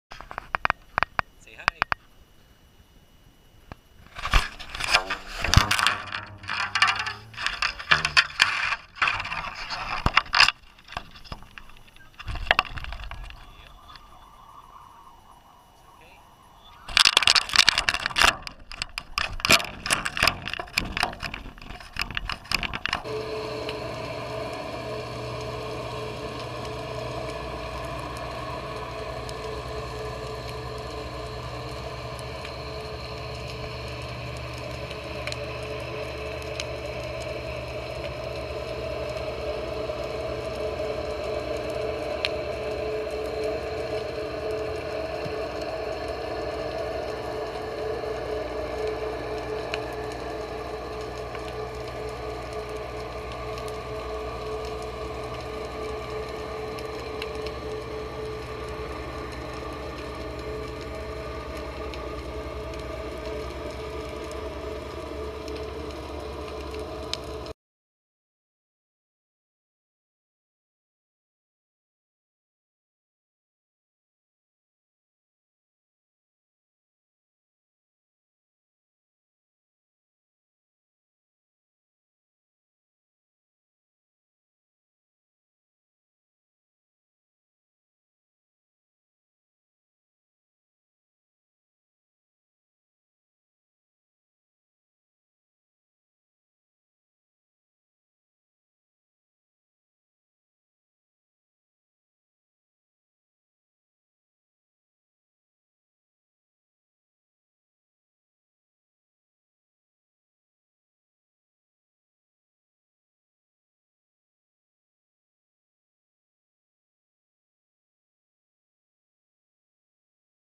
Underwater Footage at WB Pier
Took Koki fishing and had the bright idea of using one of my many fishing rigs to drop the GoPro in the water and see what was actually going on down there...I am amazed! I wanted the entire three hours of footage and most of the fish stuck around. Surprised to see no goatfish, makubu or parrot fish.